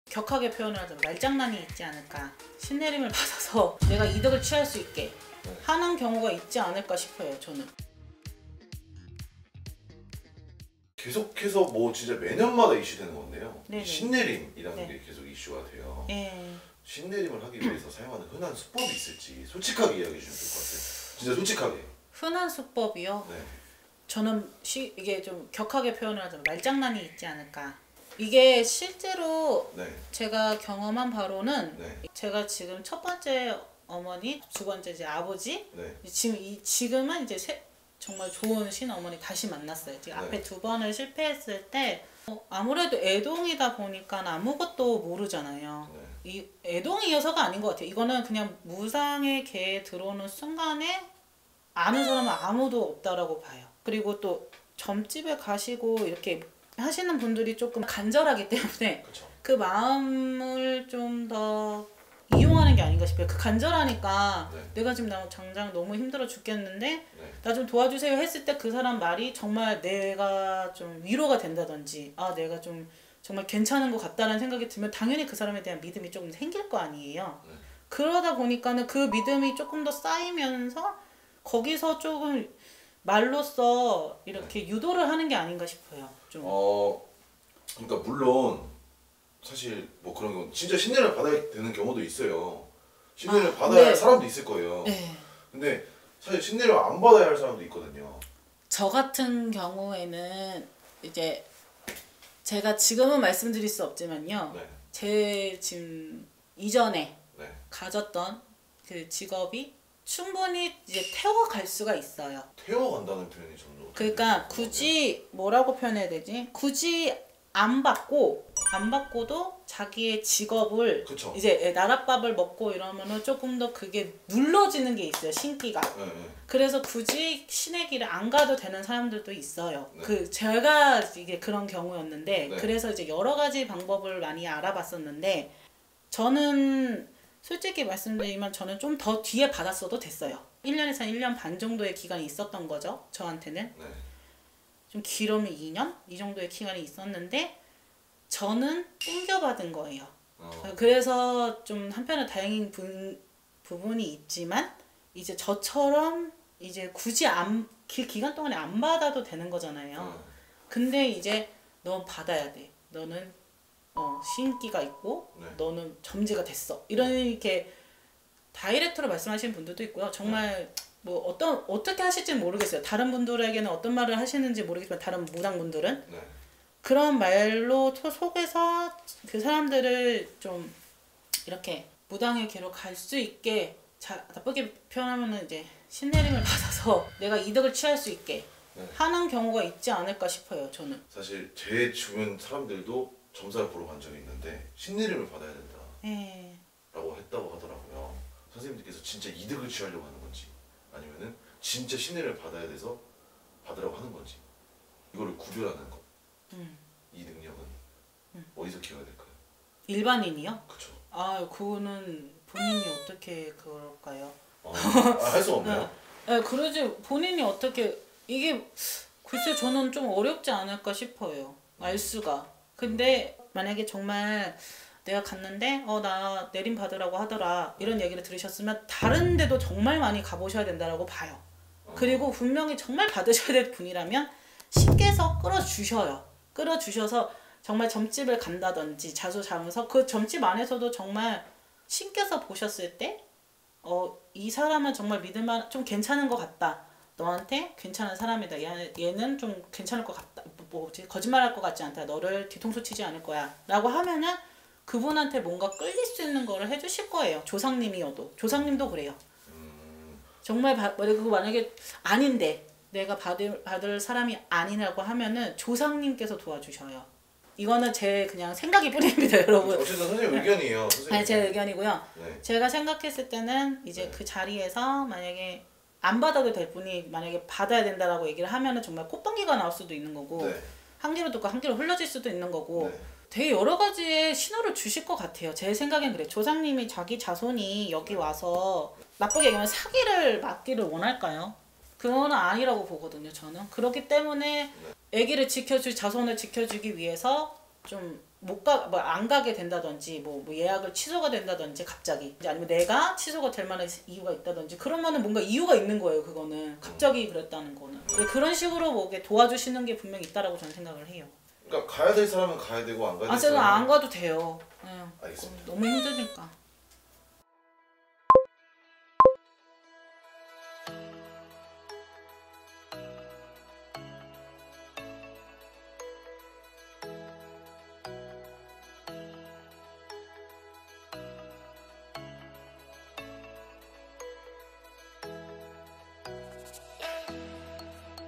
애동제자에게 물어본다! 신내림을 강요하는 흔한 수법들!!! [초롱신당]
자막

★대표블로그★
-올마이티TV